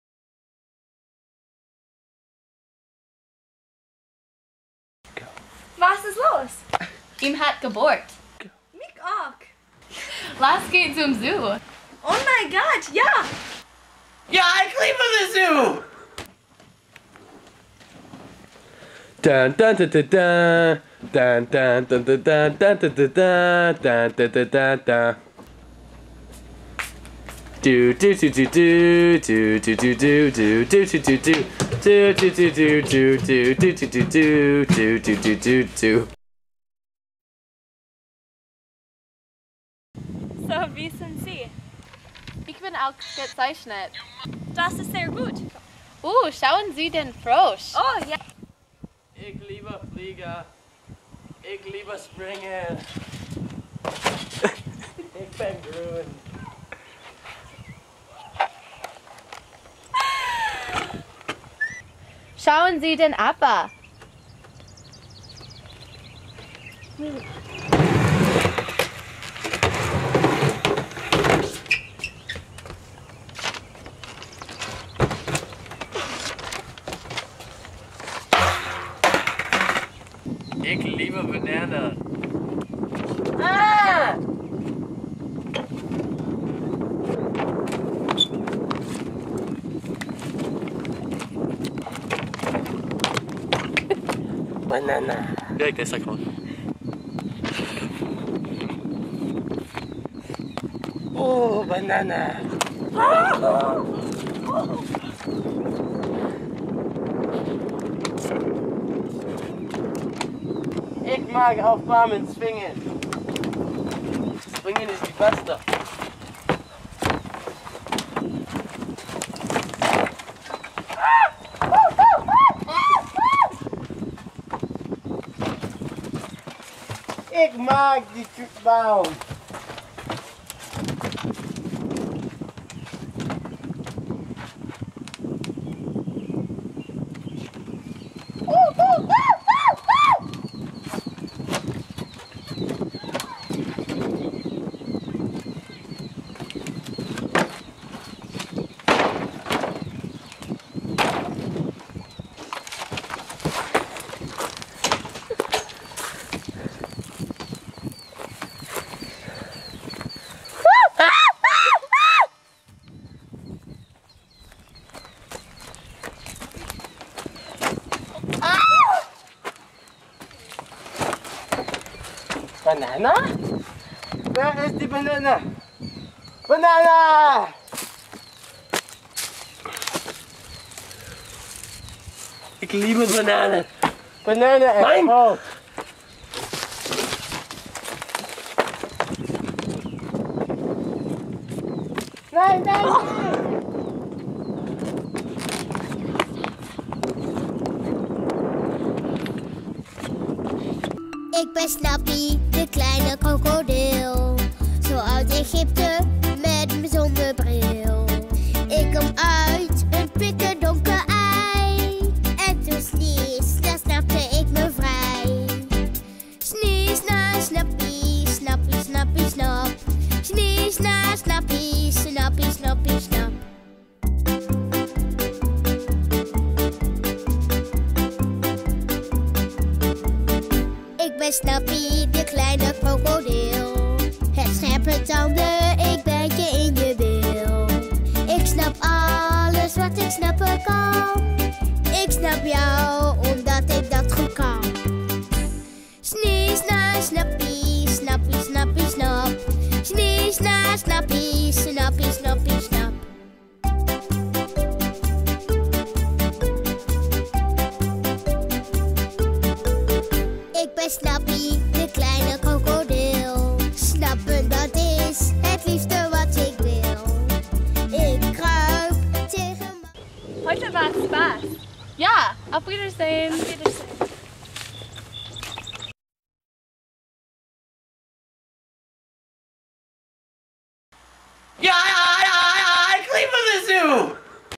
Der Miller Zoo GHS Deutsch Video
Join a couple of friends on their wonderful trip to the Miller County Zoo, where there is no telling what you'll see! Gorillas,frogs and alligators OH MY!